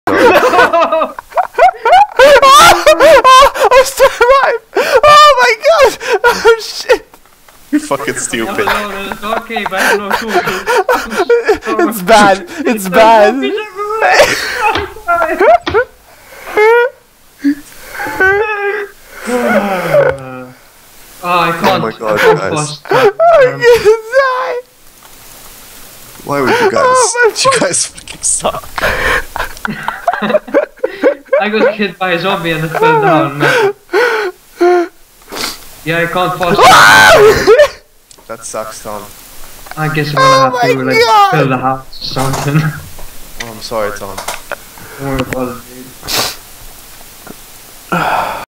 Cave Explorers
The bois go cave exploring